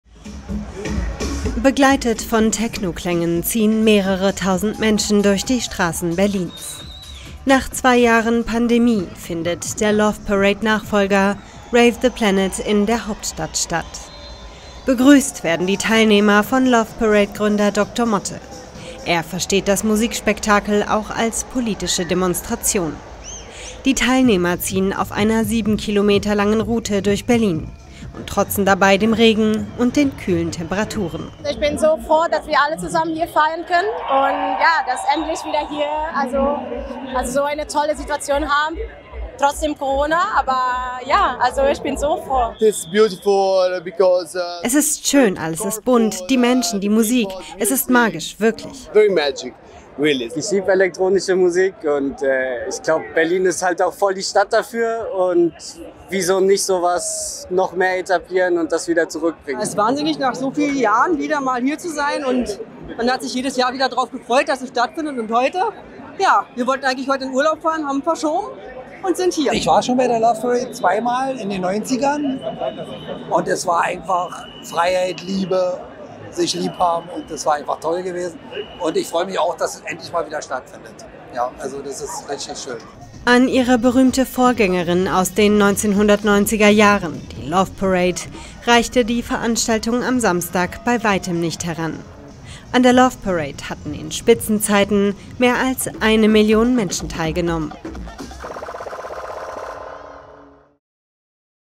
Tausende tanzen in Berlin bei Loveparade-Nachfolger Rave The Planet | AFP
Begleitet von Technoklängen ziehen mehrere tausend Menschen durch die Straßen Berlins. Nach zwei Jahren Pandemie findet der Loveparade-Nachfolger Rave The Planet in der Hauptstadt statt.